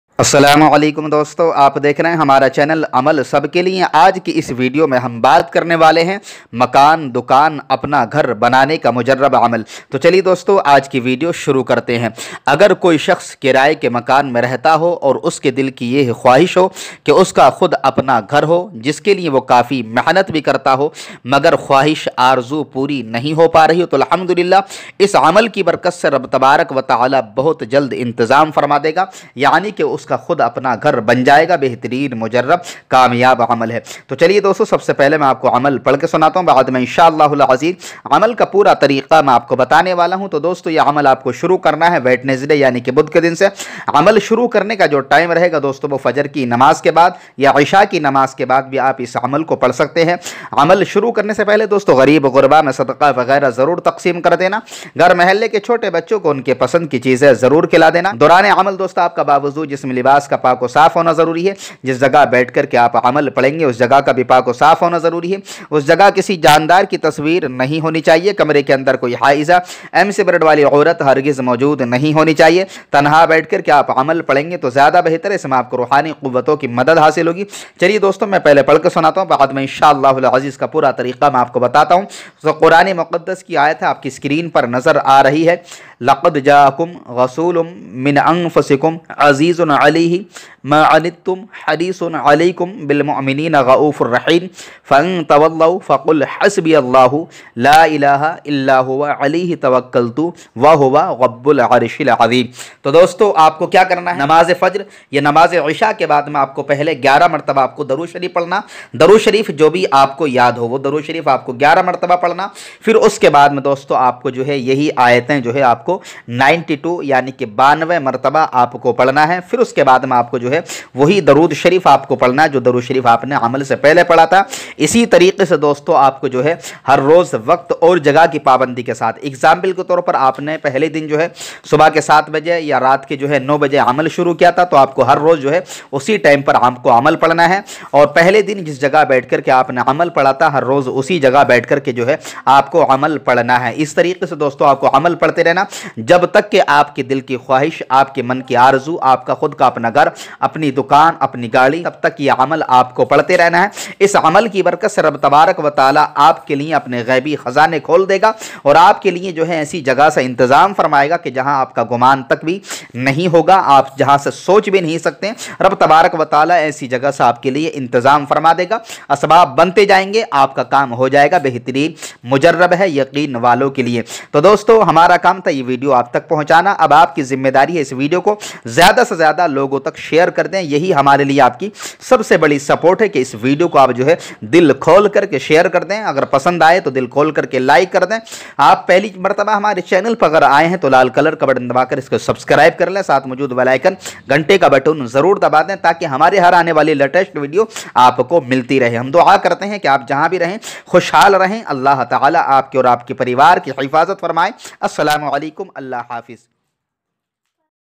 Har Hajat Pori Hone Ka Wazifa | Apna Ghar Banane Ka Wazifa
AMAL SABKE LIYE
is video mein bataya gaya hai agar aap Aaj Tak Kiraye ke Makan mein rehte hain aap ka khud ka Apna Ghar Nahi Hai Aap pareshan hai kab chuke hain kiraya De De kar Sharab Ke Fazal se Saman ki Barkat Hai Allaha Ne Chaha Toh Kuch Bhi Nahi ke andar aapka Khud Apna Makan ban jayega Har hajat aapki Puri ho jayegi

About: Me Molana Muhammad Furqan Raza Qadri...